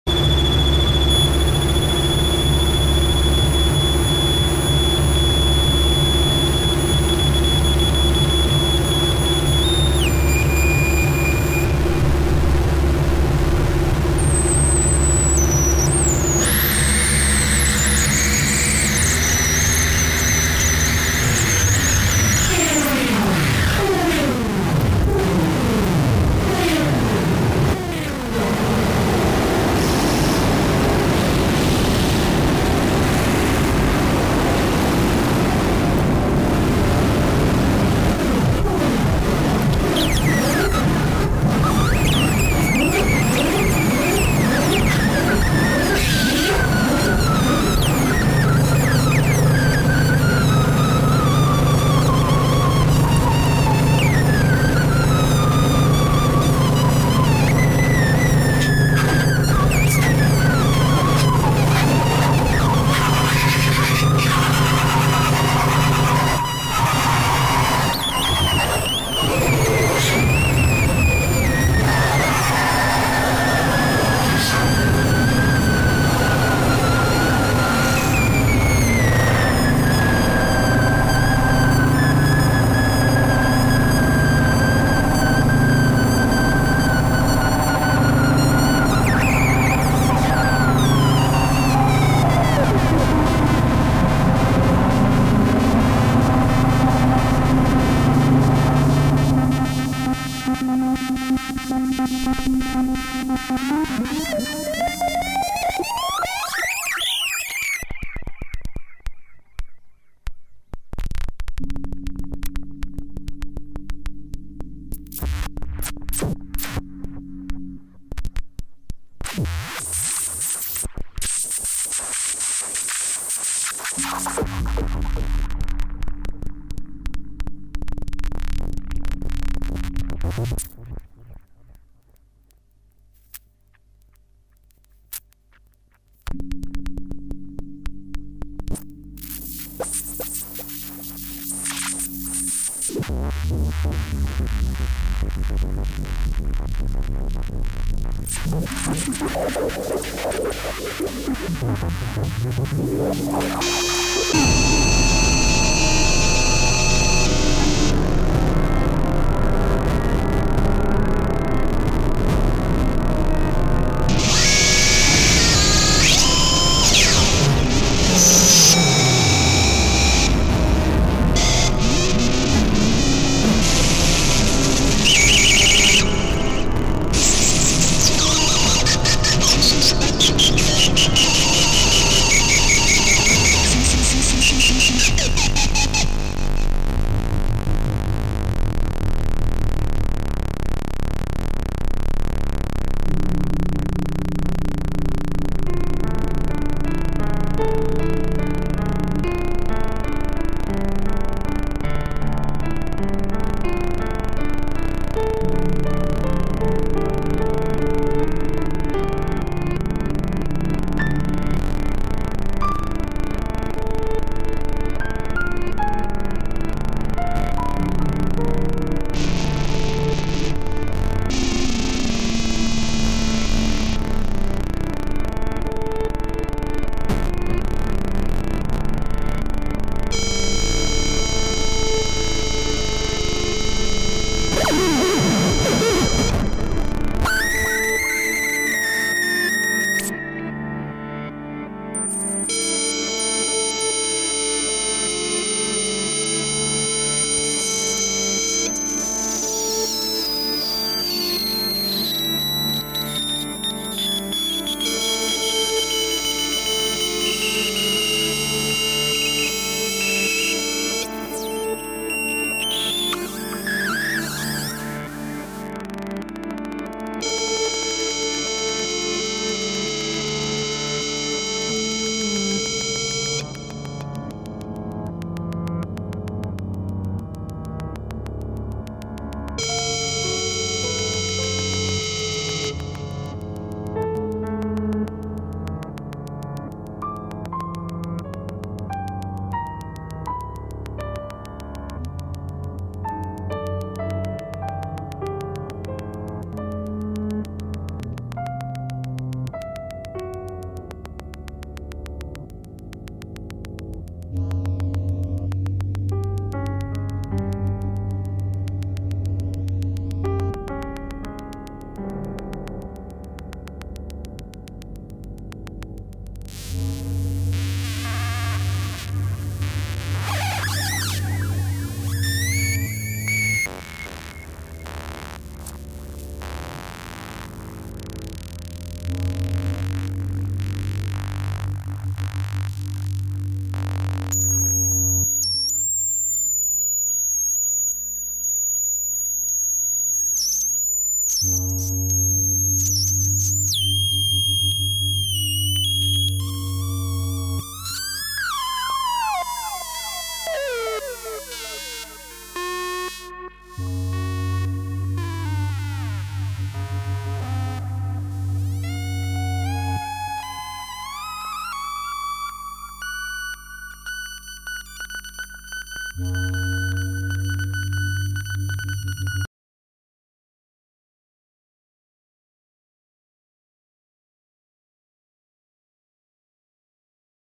psychedelic circuit bent light music 4
some circuit bent instruments, with effects added: a red 8bit synthesizer, handbuilt by michael rucci run through a noise-effectmachine, controlled by body conducts, built by drmoonstien - both running through the memory man delay device (electro harmonix), another red synth, handbuilt by vcs3dreams run through the alesis philtre, roland gaia synth, and a yamaha stagepiano combined with korg kaos pad..